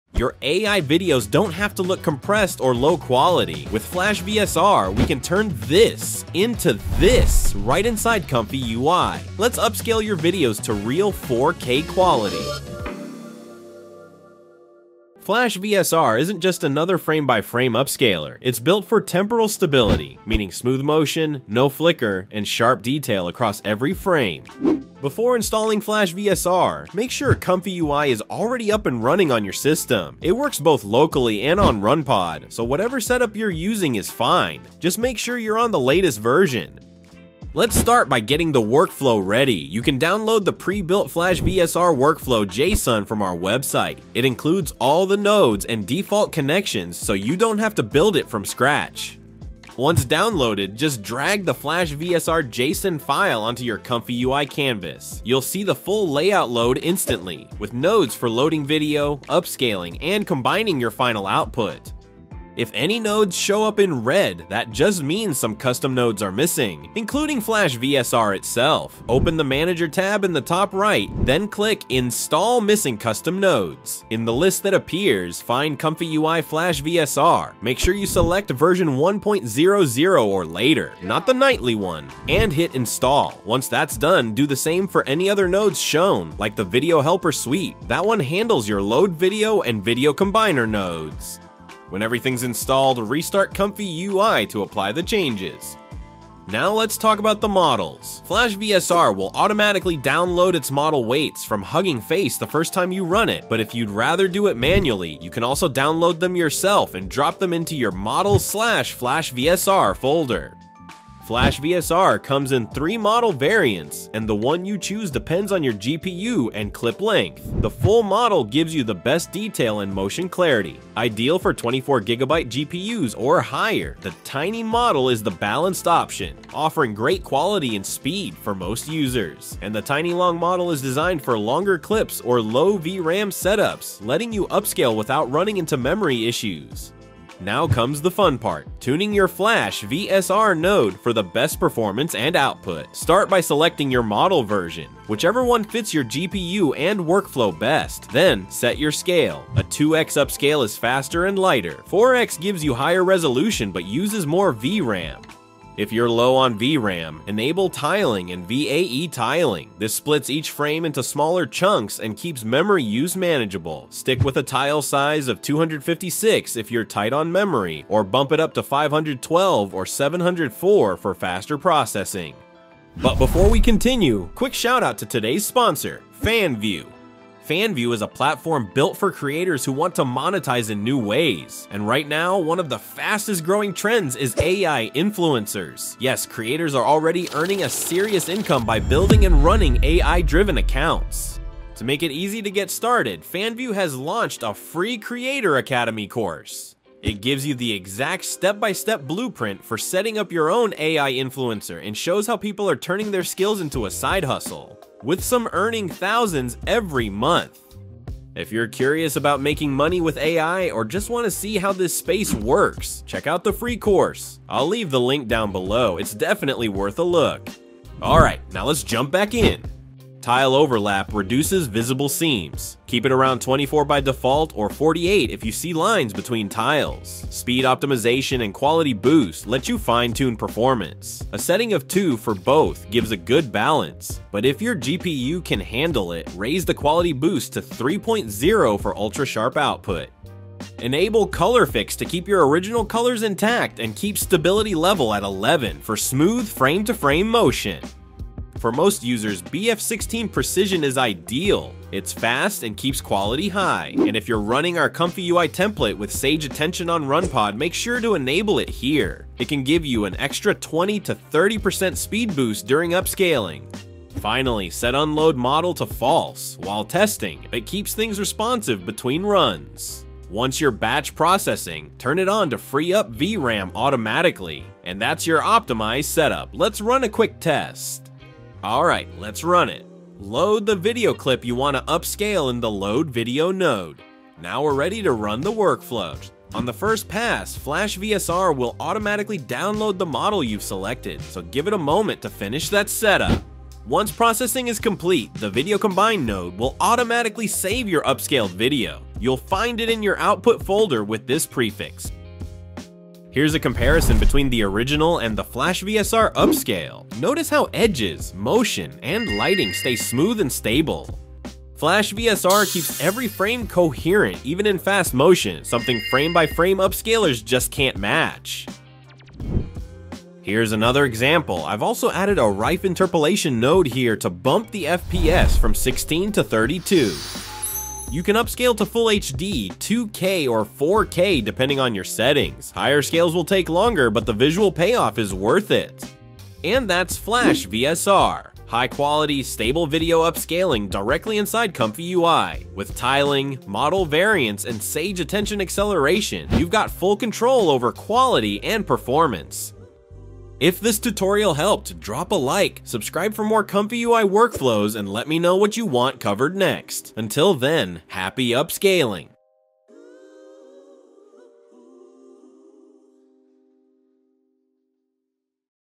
How To Upscale ANY Video To 4K With FlashVSR in ComfyUI
Upscale any video to real 4K using FlashVSR inside ComfyUI. In this tutorial, I’ll show you how to install it, set it up, and get the best quality from your clips. Watch how FlashVSR transforms regular footage into sharp, detailed 4K results with smooth motion and clean frames.

Load your first $10 and get up to $500 in credits!

🎬 Created By
• Editing, VFX, SFX, and Thumbnail → @NextDiffusion

⏱ Chapters
0:00 Intro
0:36 Video Upscale Workflow Download
1:02 Installing FlashVSR Video Upscaler
1:35 FlashVSR Models
2:17 Tuning The FlashVSR Node
4:38 Running The Video Upscale Workflow
5:03 Examples 4K Upscaled Videos
5:50 Subscribe if you read this